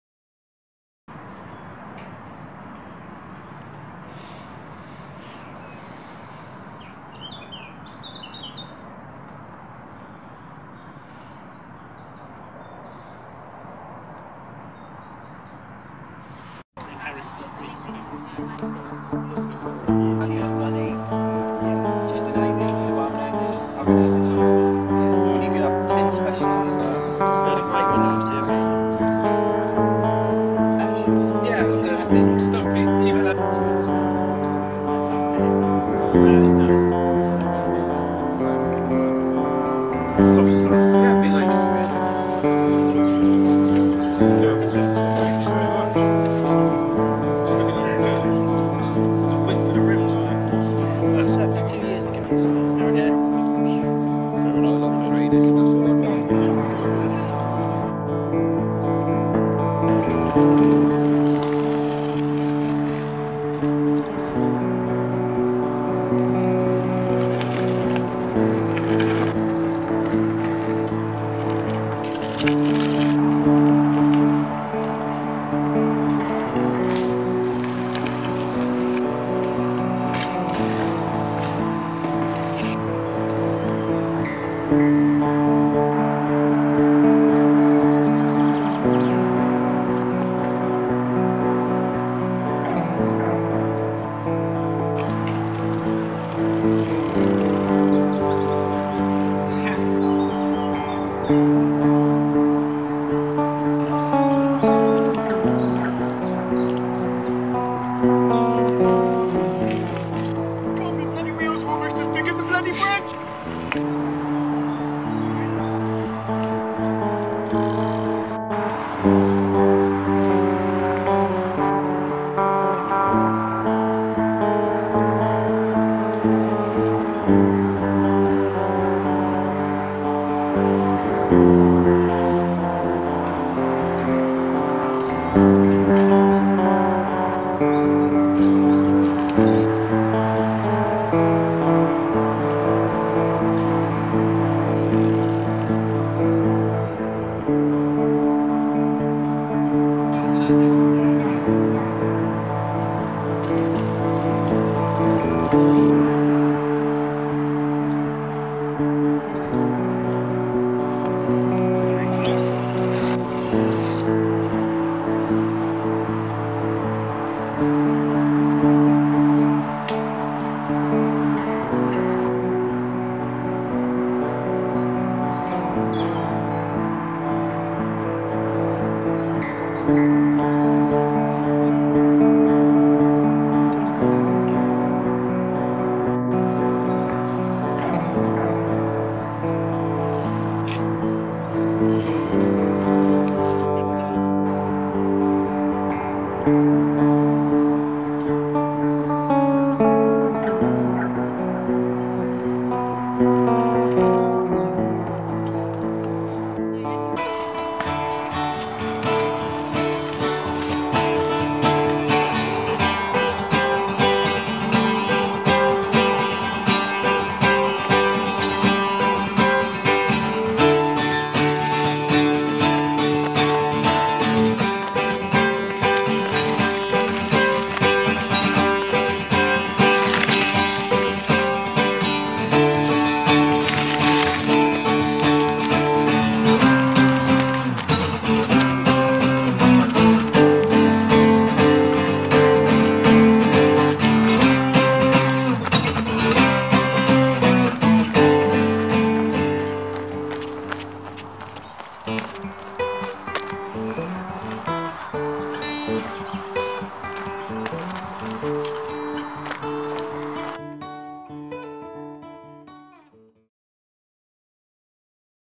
Hargroves Cycles demo day 2014
The Hargroves Cycles demo day took place at the Croft Trail in Swindon. The event was busy right from the start with bikes being taken round the trail all day.
The weather was great again and the trail was in top condition, a credit to those who work at the build days. The MBSwindon barbecue was busy all day thanks to Pete and Kat. The last visitors ended up with multi packs of burgers wrapped in foil as a bonus.